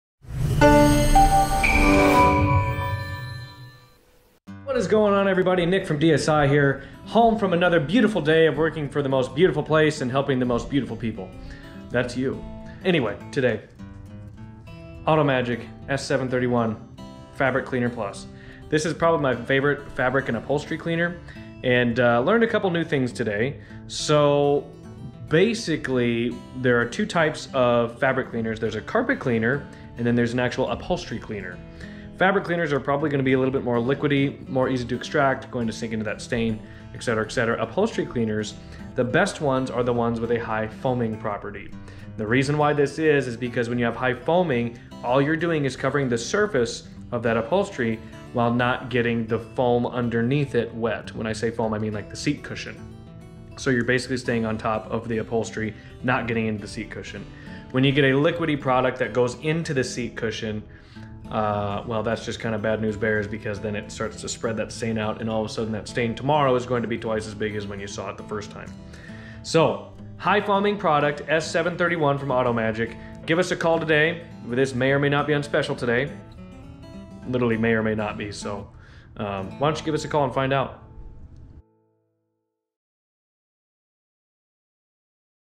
Auto Magic S731 Fabric Cleaner Plus Aerosol
Fabric Cleaner Plus aerosol is a superb aerosol upholstery cleaner designed for today's new automotive upholstery material.